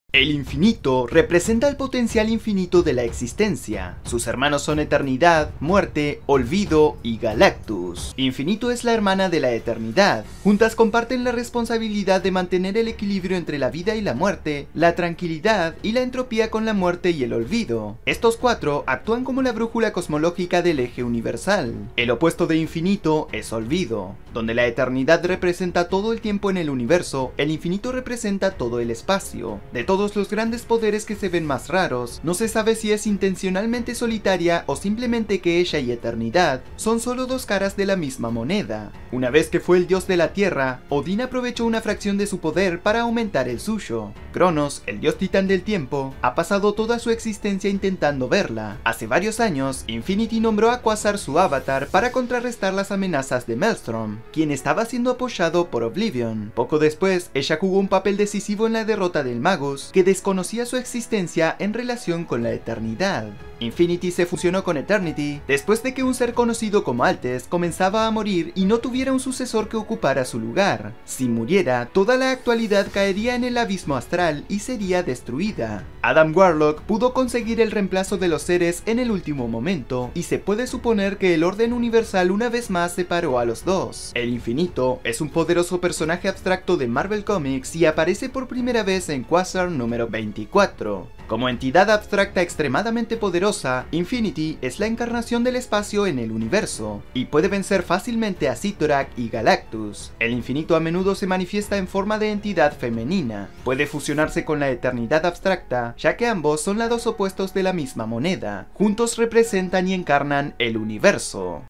La Historia De Infinty | Entidad Abstracta Que Representa El Espacio Infinito En El Universo Marvel
El infinito representa el potencial infinito de la existencia. Sus hermanos son Eternity, Death, Oblivion y Galactus.

Mis videos con más vistas!! Si al publico le gustó tal vez a ti también: